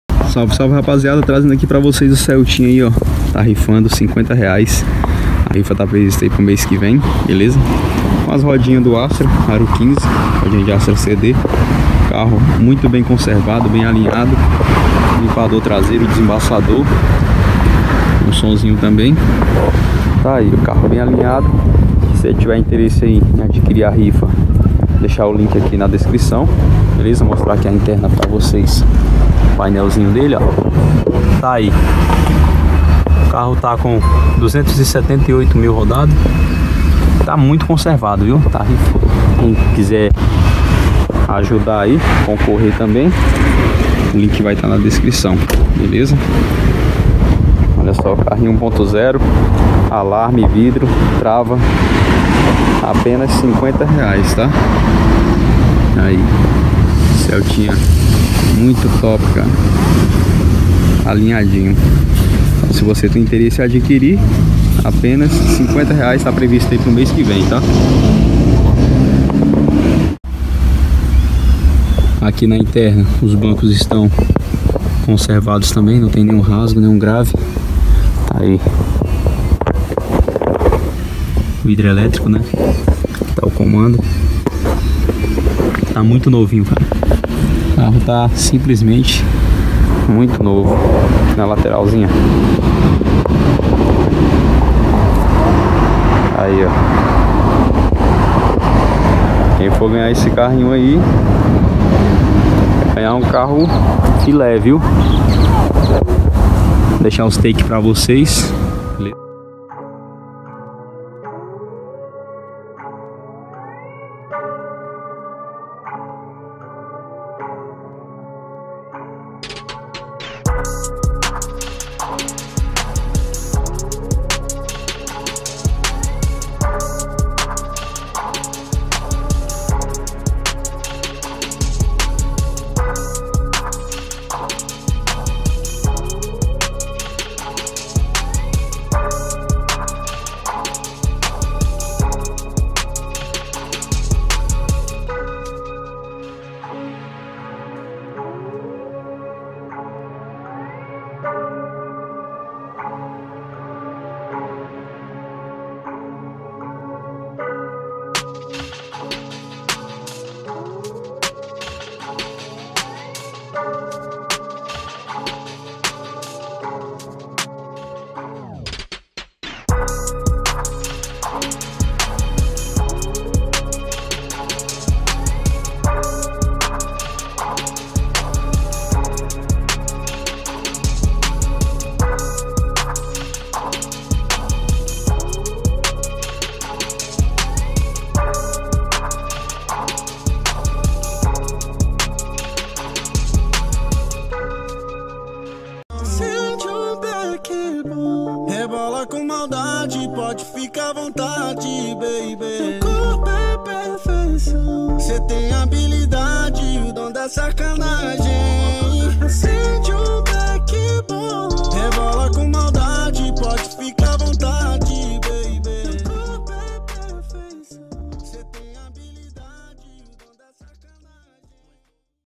CHEVROLET CELTA 1.0 LIFE 2002 POR R$50,00!
ESSE CHEVROLET CELTA PODE SER SEU POR APENAS R$50,00.
CHEVROLET CELTA 2002/2003 1.0 LIFE 2 PORTAS VHC.
OPCIONAIS:
-VIDRO ELÉTRICO;
-TRAVA ELÉTRICA;
-ALARME;
-SOM;
-BANCOS DO CELTA MAIS NOVO ;
-PAINEL DO CELTA SPORT;
-RODAS LIGA LEVE ASTRA ARO 15;
CARRO TODO EM DIA PINTURA NOVA QUEM TIVER INTERESSE EM COMPRAR A RIFA CHAMAR NO ZAP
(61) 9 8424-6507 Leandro